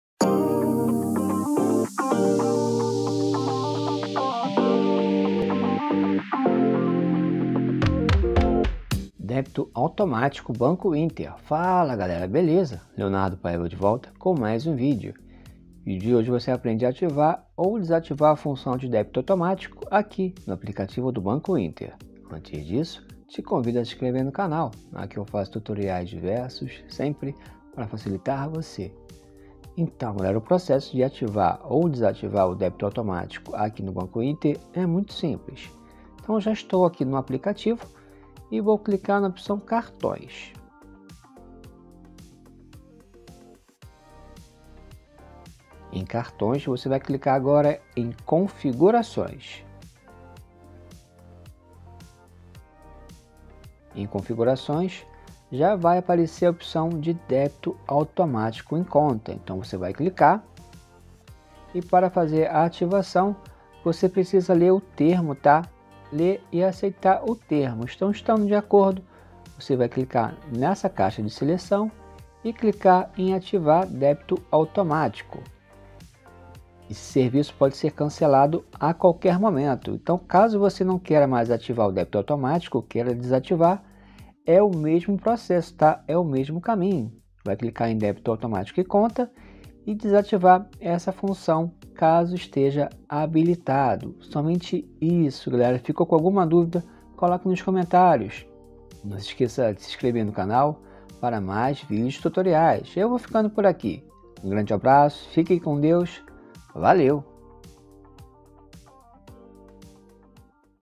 ATIVAR ou DESATIVAR Débito Automático no Banco Inter - Passo a Passo!
Vídeo Tutorial de hoje explico como ativar ou desativar a função de débito automático no aplicativo do Banco Inter.

Neste Vídeo:
Ativar ou desativar função débito automático no banco Inter

Capítulos:

00:00 Introdução
00:09 Débito automatico banco inter (Explicação inicial)
00:22 Convite para se inscrever no canal
00:29 Ativar ou desativar debito automático
01:35 Encerramento